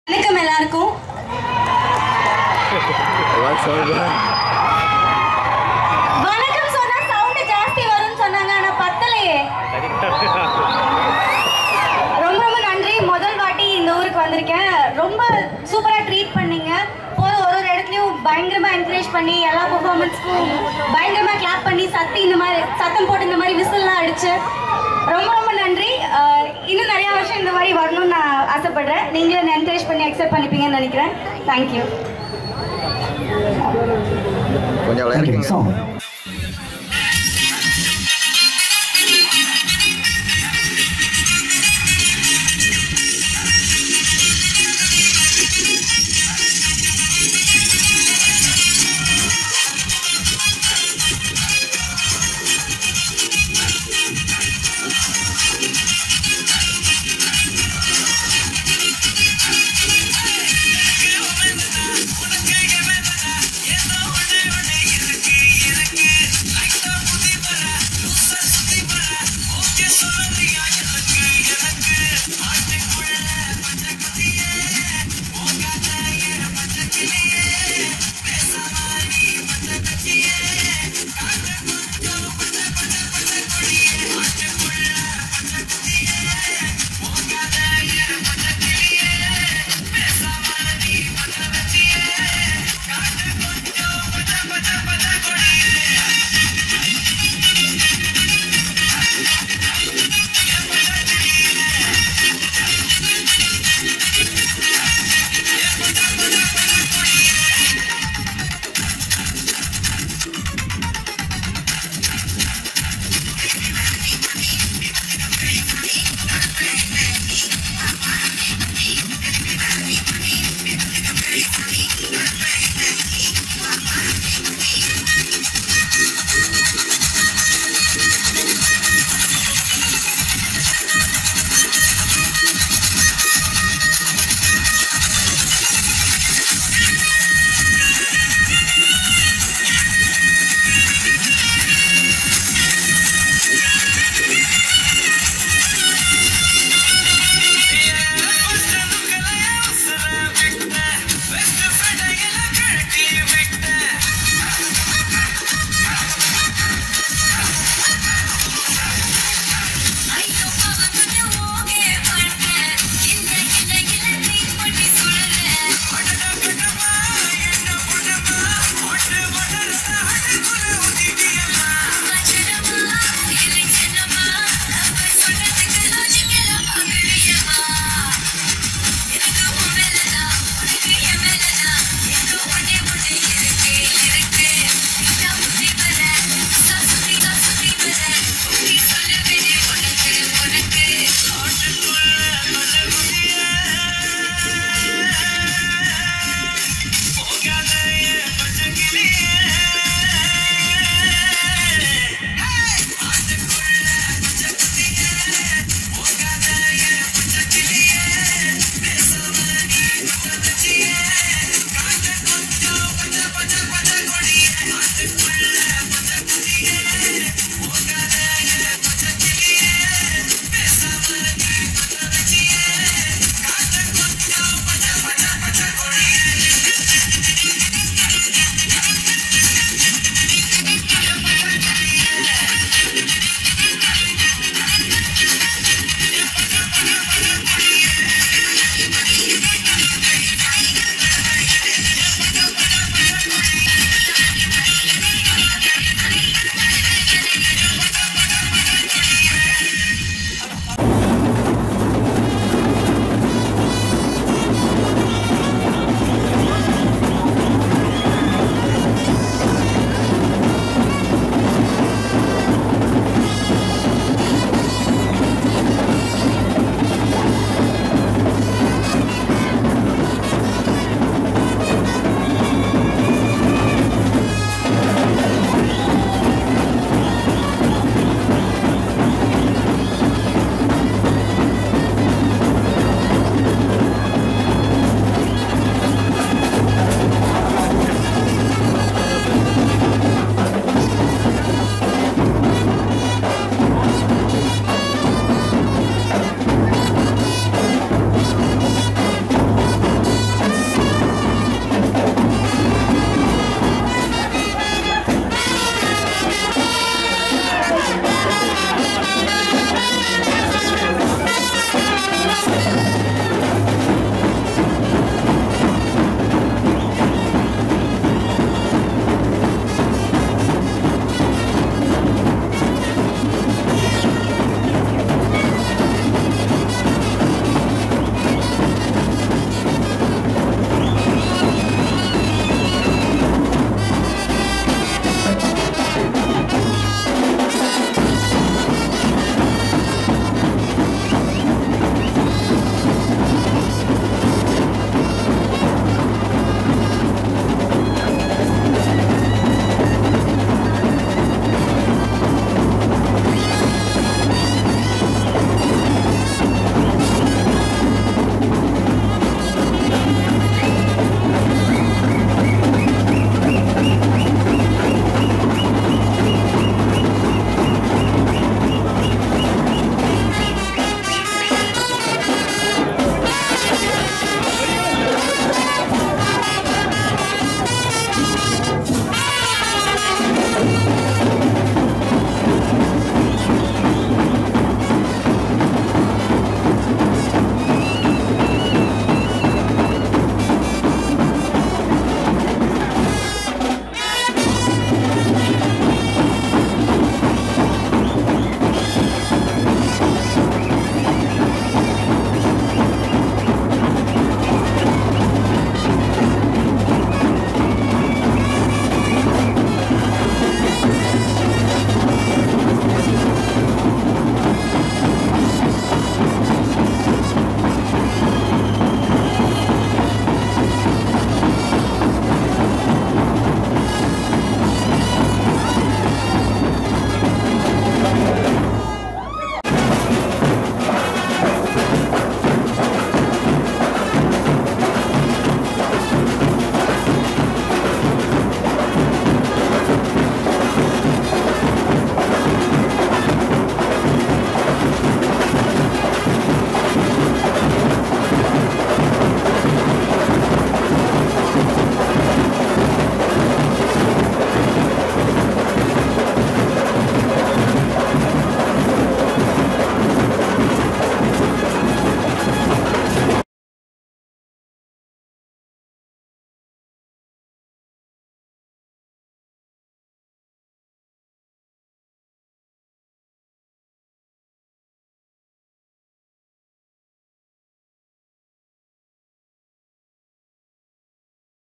Vijay Tv Cook With Comali 2 Pavithra Lakshmi Super Dance Performance On Stage in Kulasai Dasara
Cook with Comali 2 Pavithra Lakshmi Talk about Kulasai Dasara KULASAI DASARA 2020 thandavankadu dasara 2020 kulasai disco dance